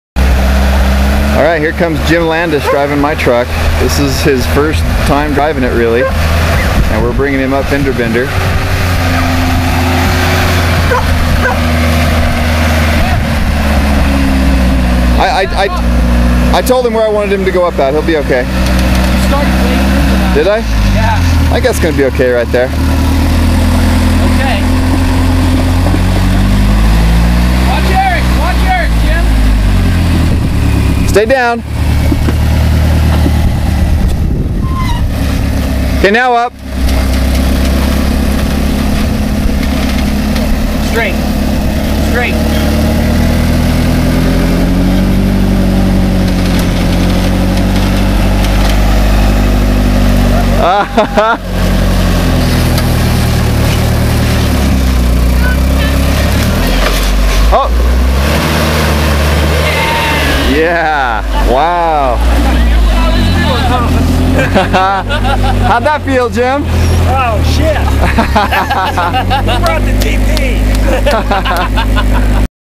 Jim Up Fender Bender
First Time Driver Up a Hard Line on Green Day in the Area BFE in Moab Ut.